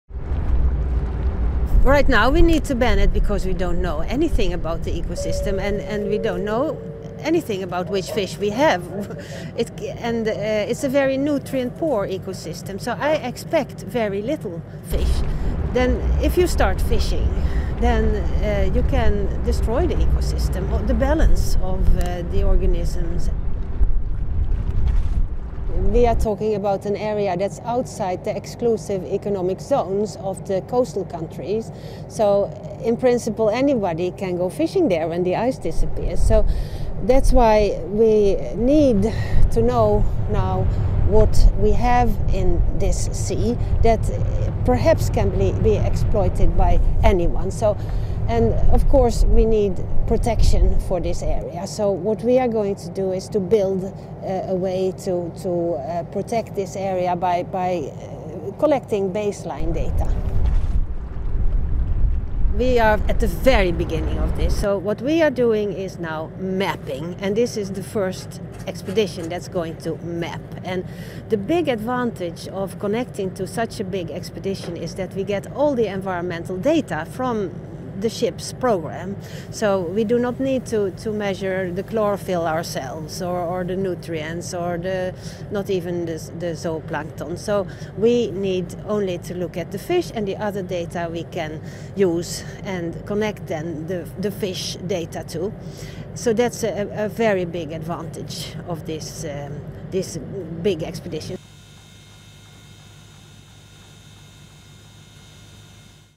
Mapping the Arctic sea life
"If you start fishing, you can destroy the ecosystem, or the balance of the organisms." This MOSAiC expedition scientist is mapping the hidden Arctic sea life…